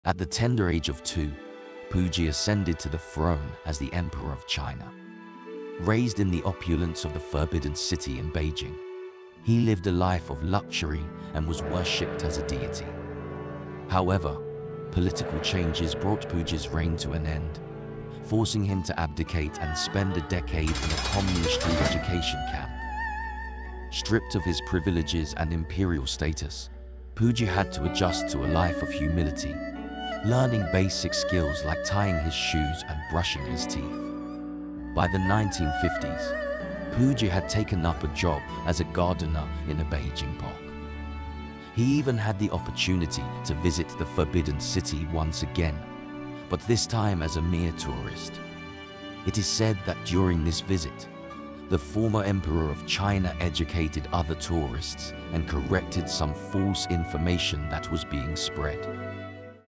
Puyi the last Emperor of China
Join us as we take a journey back in time to the life of Puyi, the last Emperor of China. Experience his rise to the throne at the tender age of two and his journey through luxury, worship, and political turmoil. Follow Puyi as he is forced to abdicate, spend a decade in a reeducation camp, and eventually adjust to a life of humility. Explore his fascinating story through the 1950s as he takes up a job as a gardener and visits the Forbidden City once again. Don't miss this captivating tale of the life of one of China's most memorable figures.

Author
Author
G41rn8